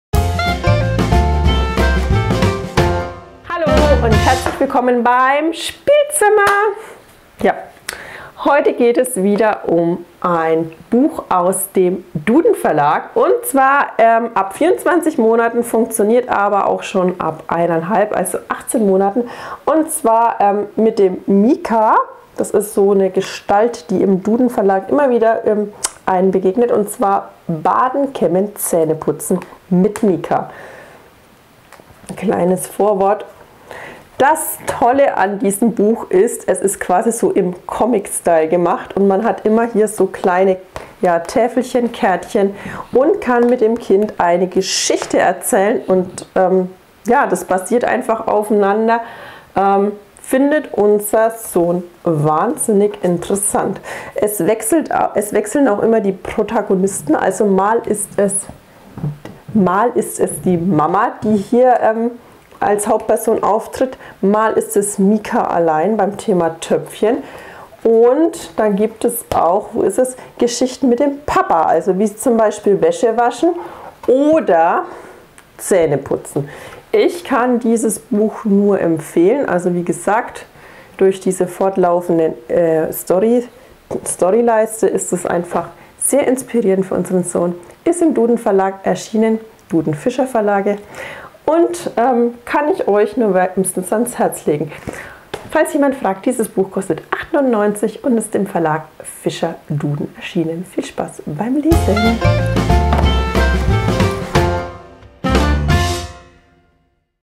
DUDEN Verlag Baden, Kämmen, Zähneputzen mit Mika | Ein tolles Bilderbuch im Comicstyle
Ein tolles Kinderbuch aus dem Verlag DUDEN Fischer Verlage ist das Buch Baden, Kämmen, Zähneputzen. Es ist im Comicstyle gehalten und der kleine Mika lernt spielerisch was Körperpflege bedeutet.

Tolles Lernbuch welches die Kinder zum selbst Anschauen inspiriert.

Selbstbezahltes Werbevideo.